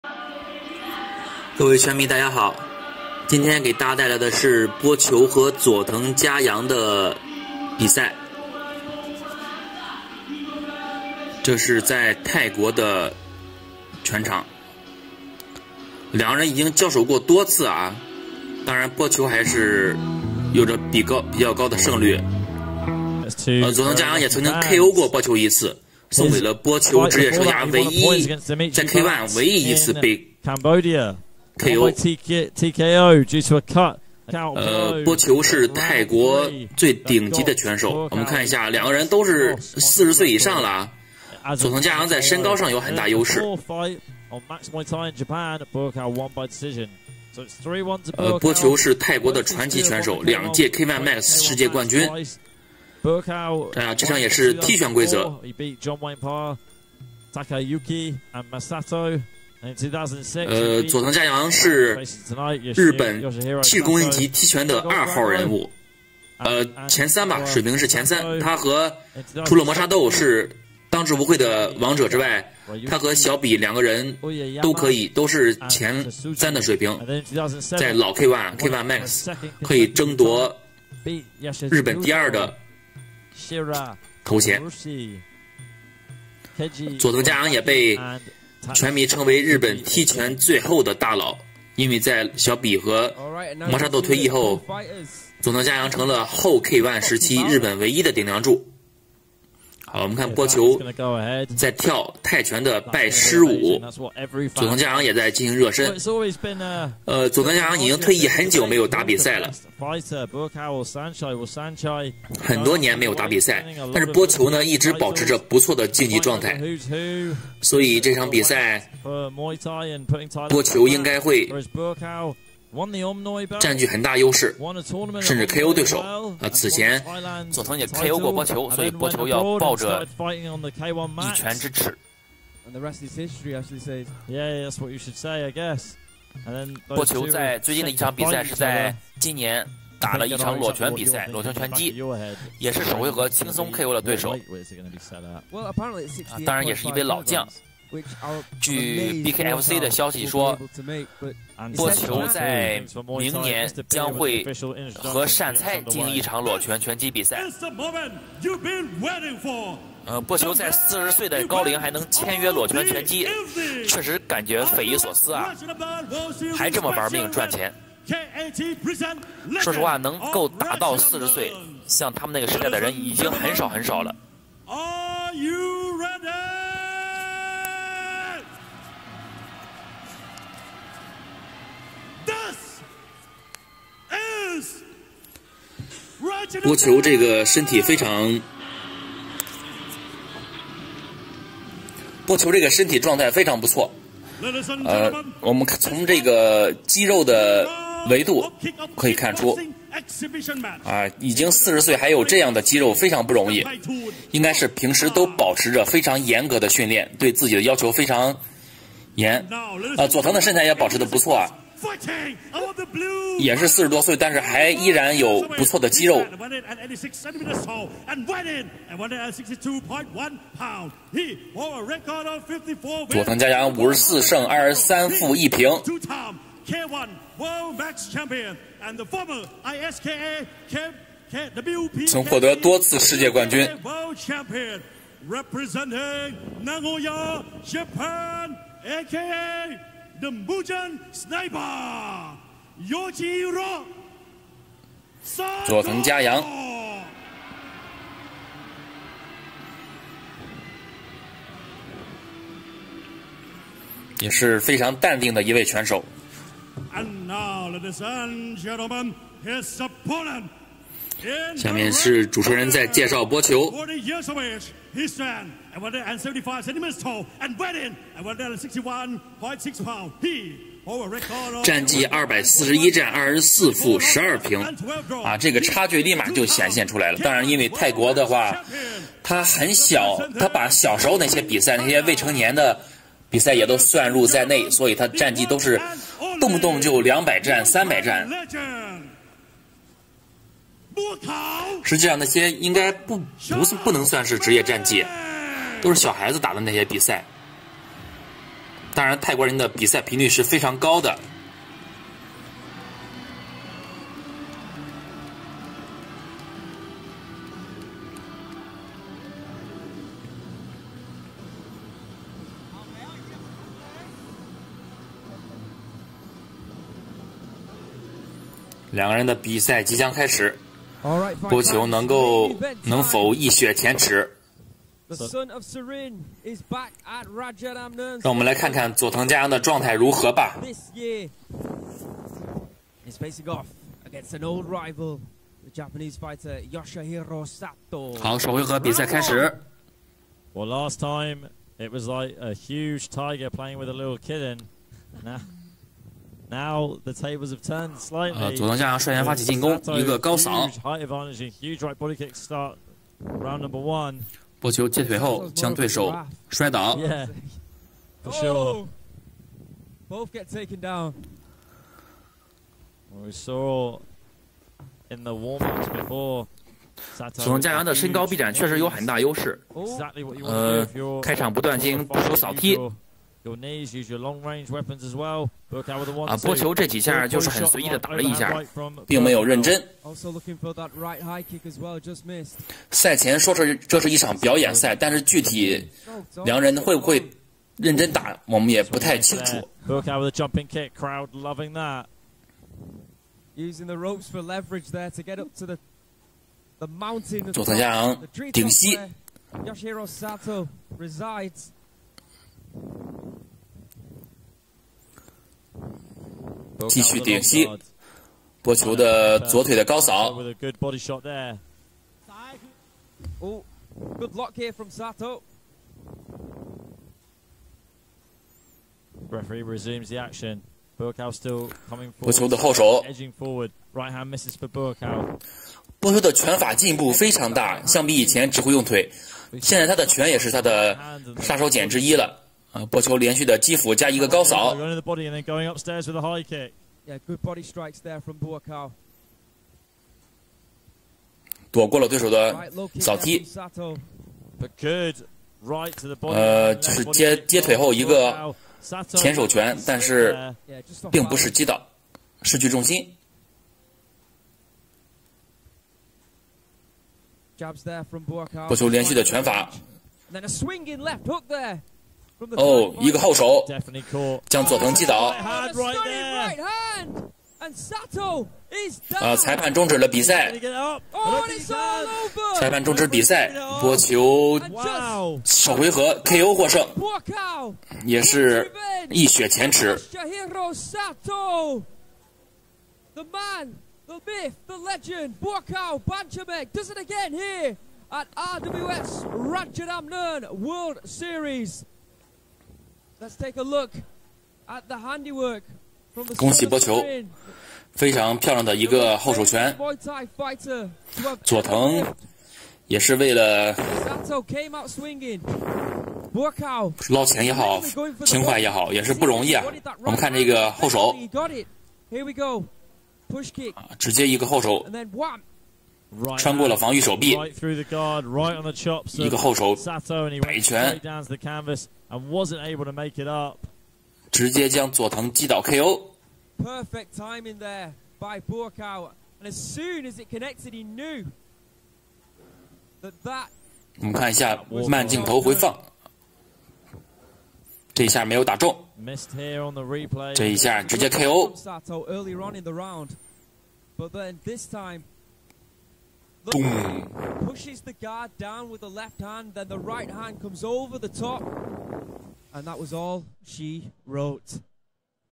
40歲播求最新比賽，一拳KO日本大佬，終於報仇雪恨！| Buakaw vs sato
播求 vs 佐藤嘉洋